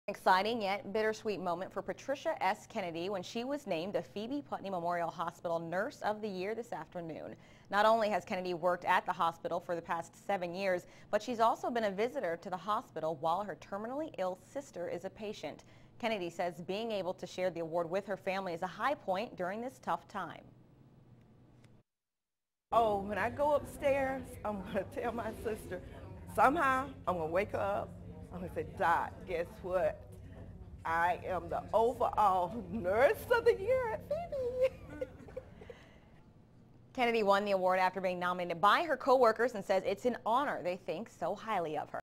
Phoebe Putney names its Nurse of the Year
Phoebe Putney Memorial Hospital named registered nurse Patricia S. Kennedy as their nurse of the year on Thursday afternoon during an awards ceremony.